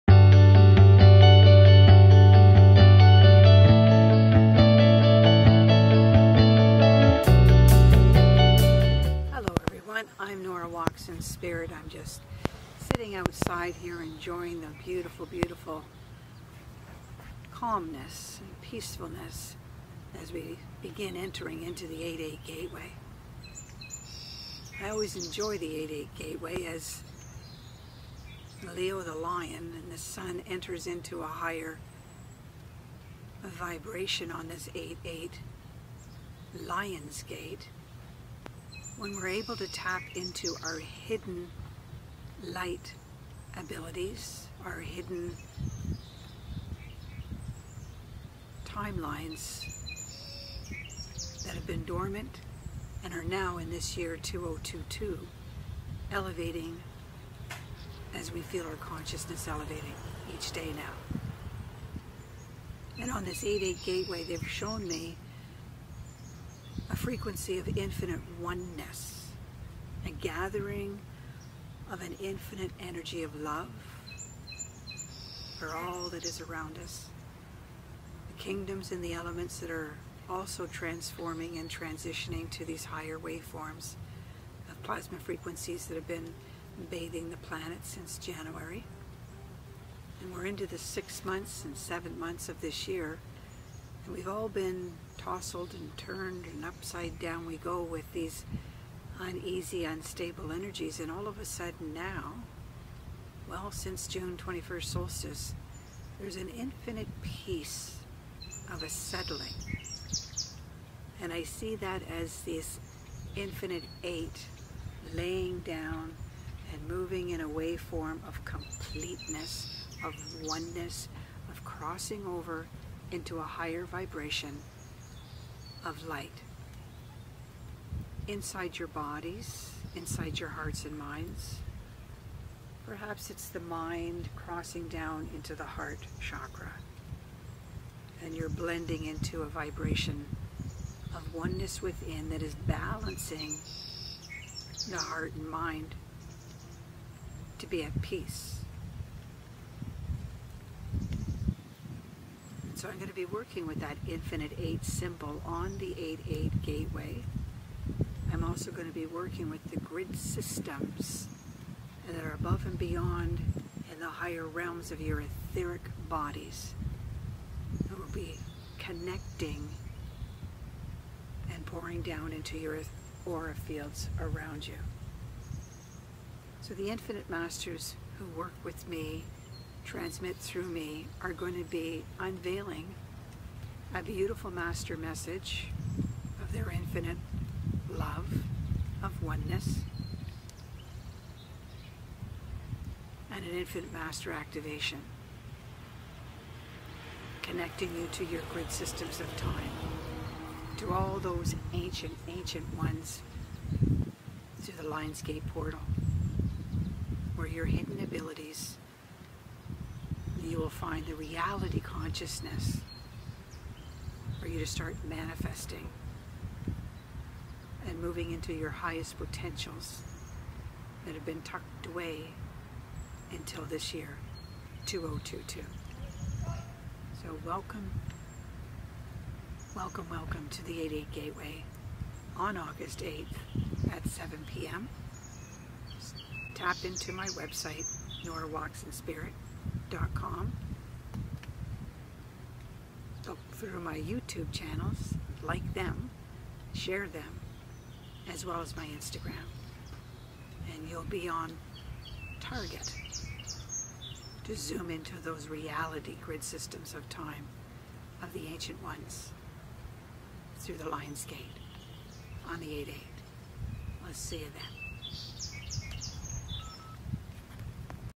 8:8 Gateway is almost here!
I welcome you to join ME for the 8:8 LionsGateway Energies that are flowing upon us.  This Portal of Divine Light opens our hearts and minds to a vibration of ONENESS of Love.  The Infinite Masters will be activating a Infinite 8 Portal within all Hearts on this 8:8 Gateway